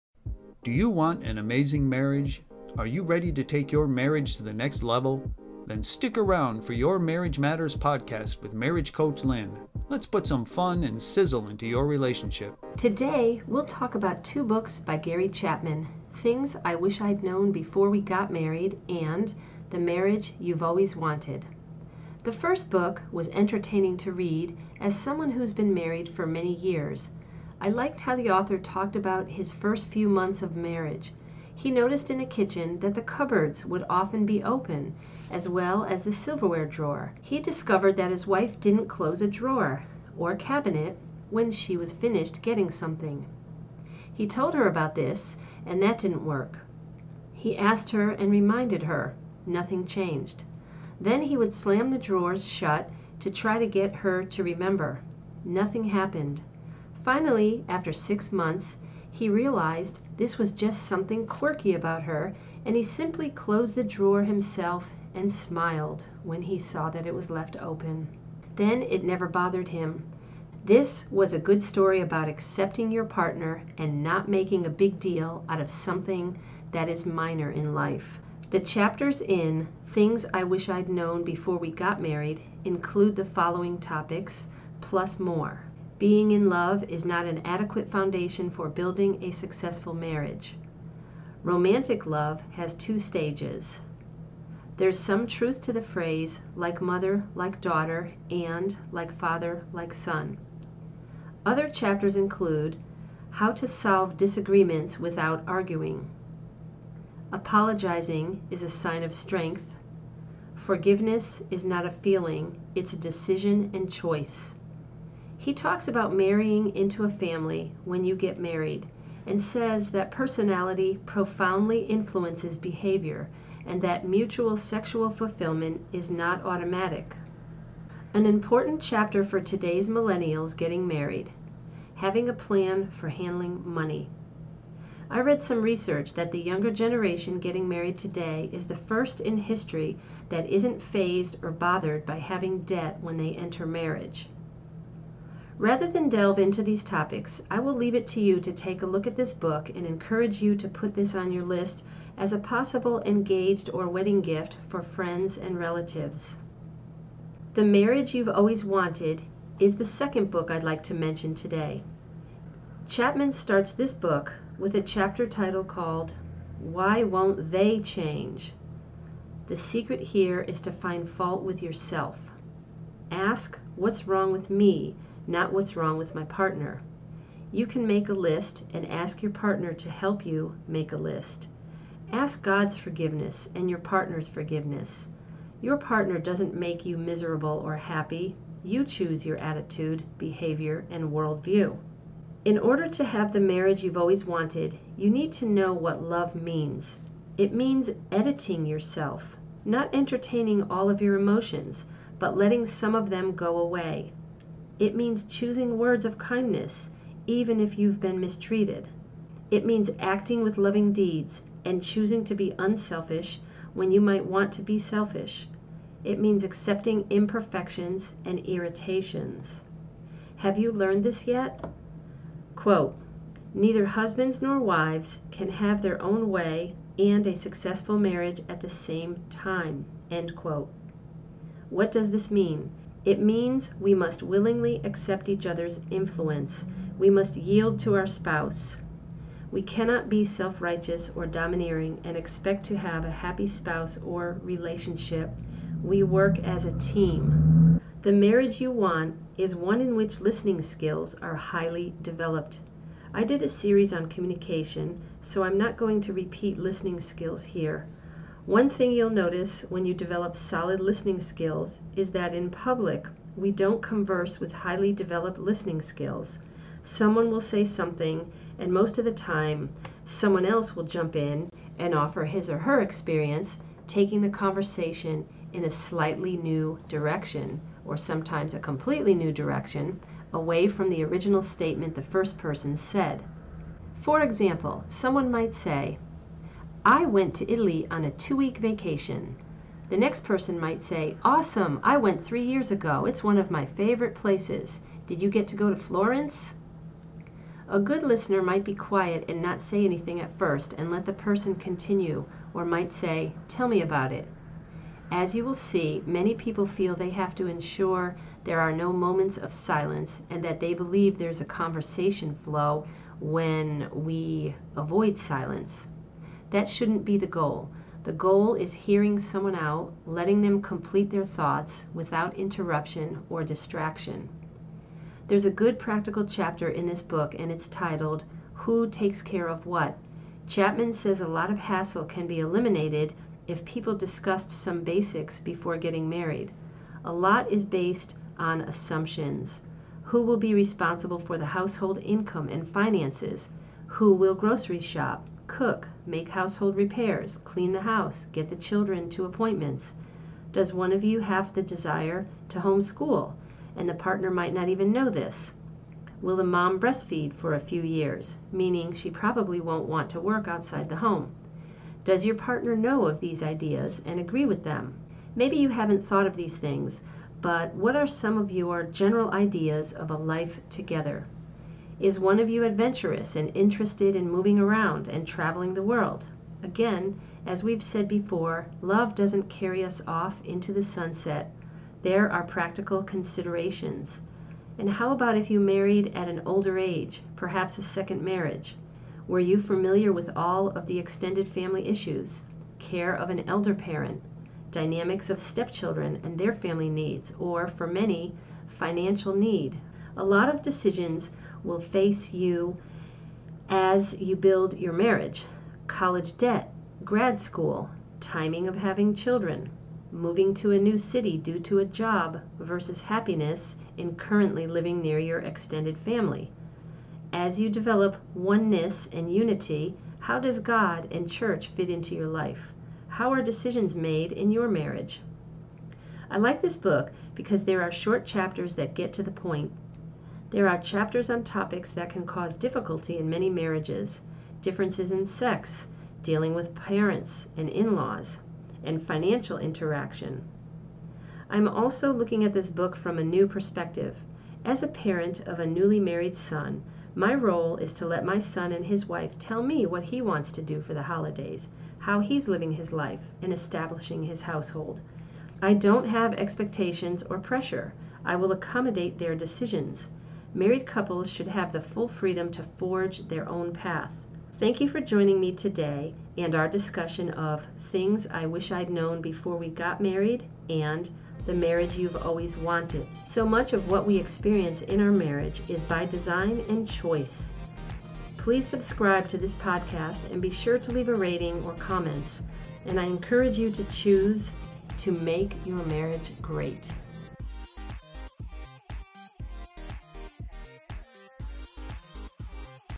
The Marriage You've Always Wanted - Podcast Episode YMMP063
2 books will be discussed today: “Things I Wish I’d Known Before We Got Married” and “The Marriage You’ve Always Wanted.” Do you have the marriage you’ve always wanted? Do you know what’s going wrong? Have you gotten off track? There are some simple areas to pay attention too. Let’s take the mystery and frustration out of relationships and focus on what matters.

- You're invited to join the Your Marriage Matters Movement if you support lifelong, happy marriage.